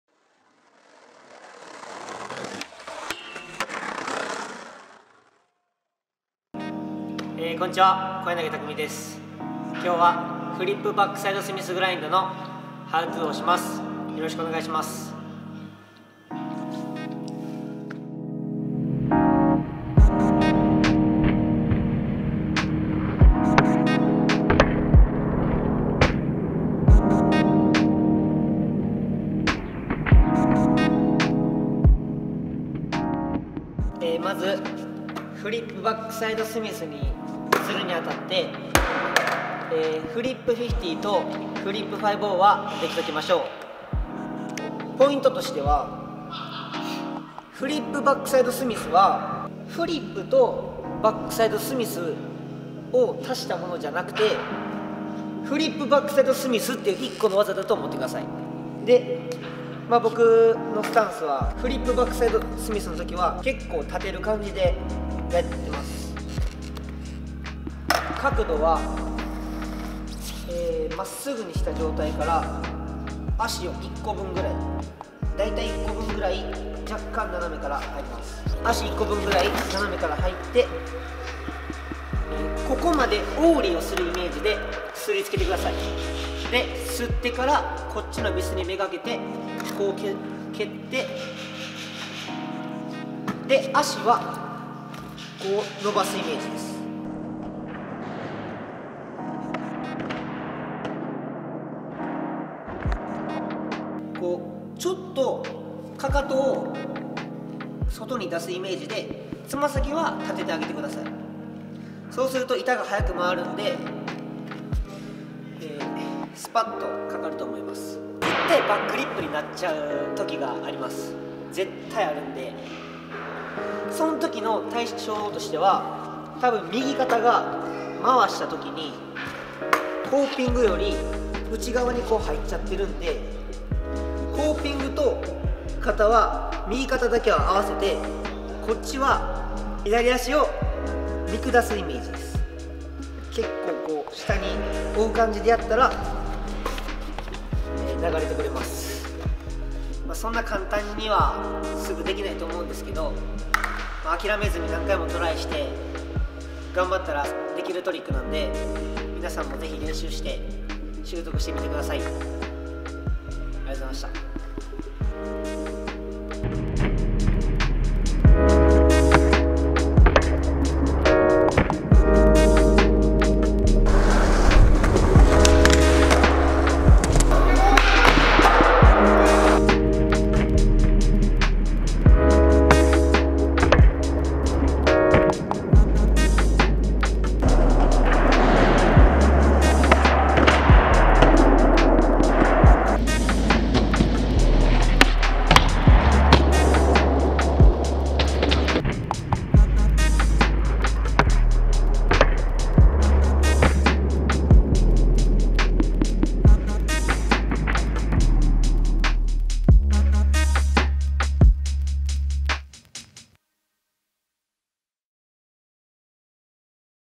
ワンランク上へ！小柳タクミが「フリップバックサイドスミスグラインド」教えます。
これが出来ればワンランク上へ！ ってことで人気スケーター達の得意技を伝授します。
今回は小柳タクミがフリップバックスミスを解説。
フリップ足の抜き方や肩の入れ方など、実践的なアドバイスが盛りだくさん。